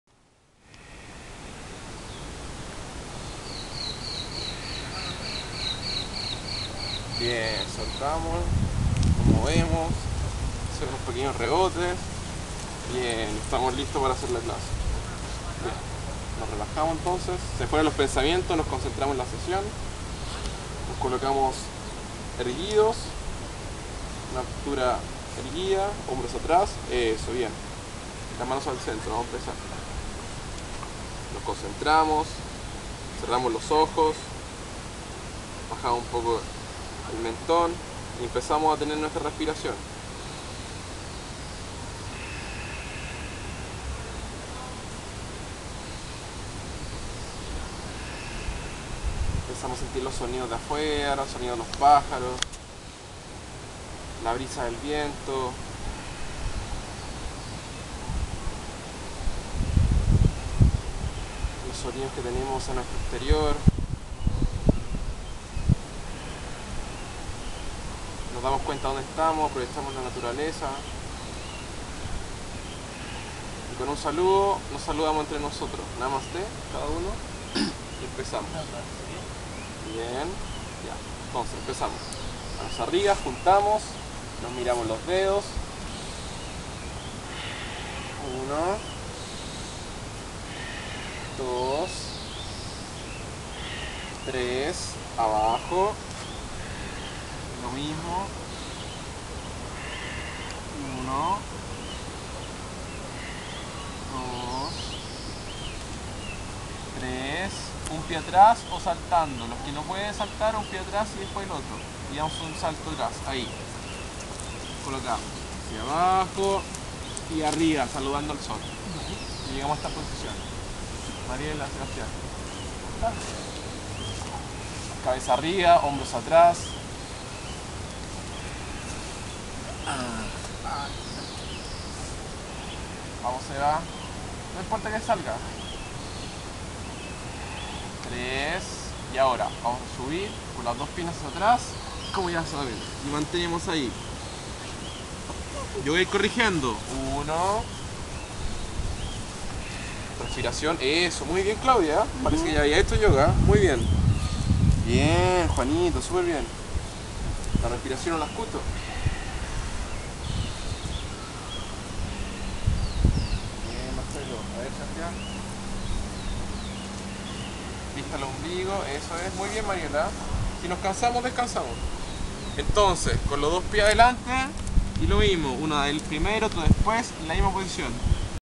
Yoga and Pilates with Down Syndrome students II
Classes made for my Down Syndrome students, Special need students requirements are not so different as regular students, even with their physical and cognitive obstacles with a good pedagogy, good plan of classes  and dedication they can improve their physical and cognitive skills. Physical activities and sport are made for everyone in any environment.